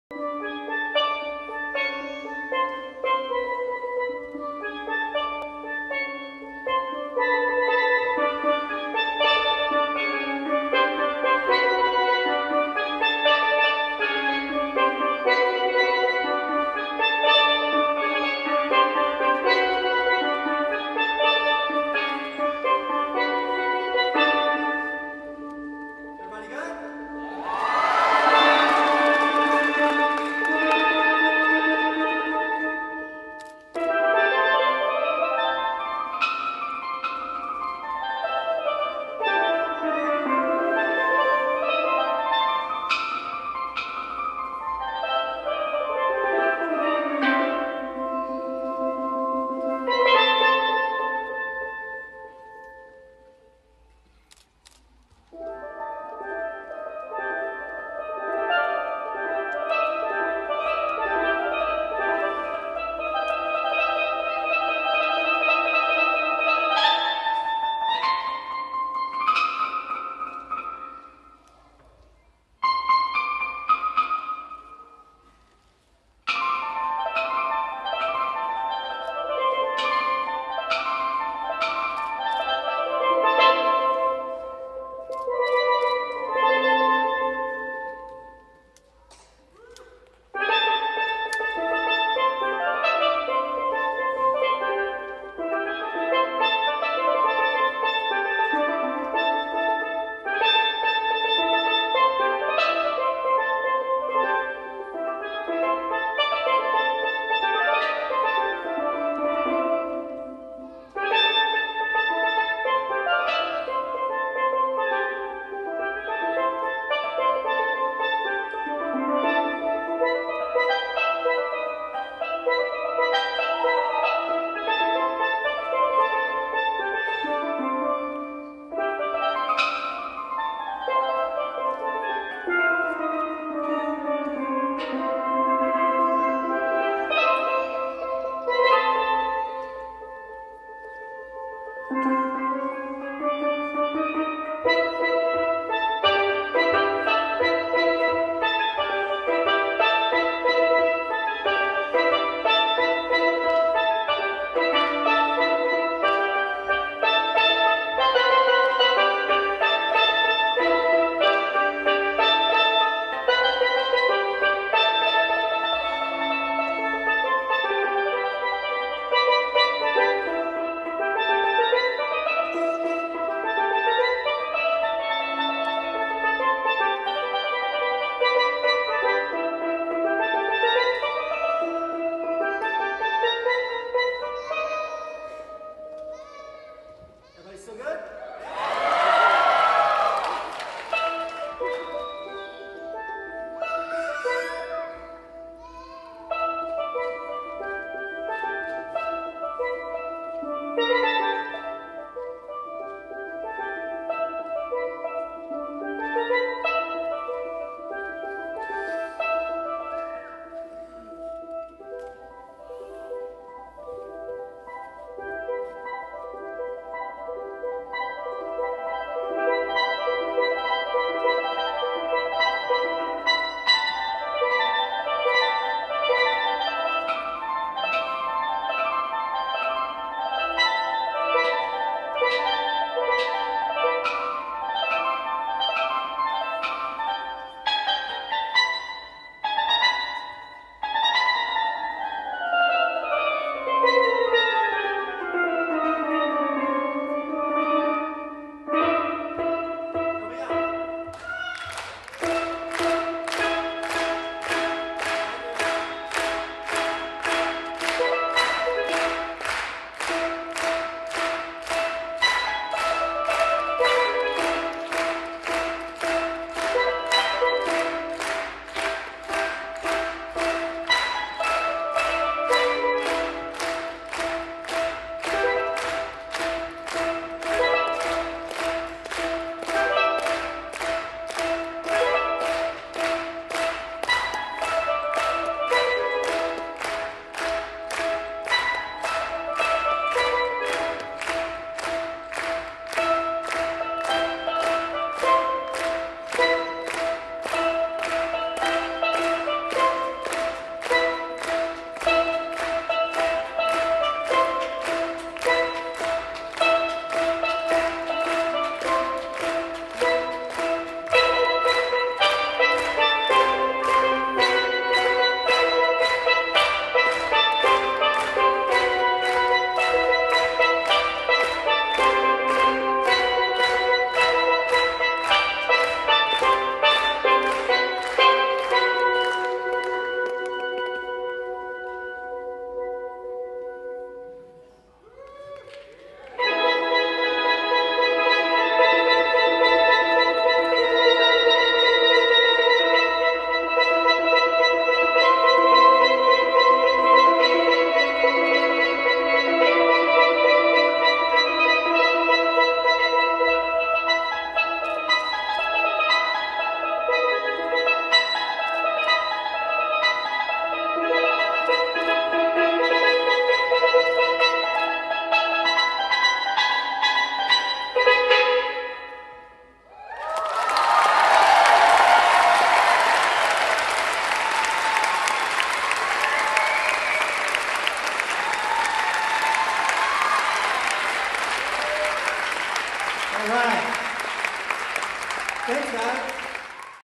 Tracy Thornton "Pan Rocks" Pan Solo 2013
Pan Rock's Tracy Thornton performing his steelpan solo @ The "Pan
Rocks" concert in Akron, Ohio, November 9, 2013.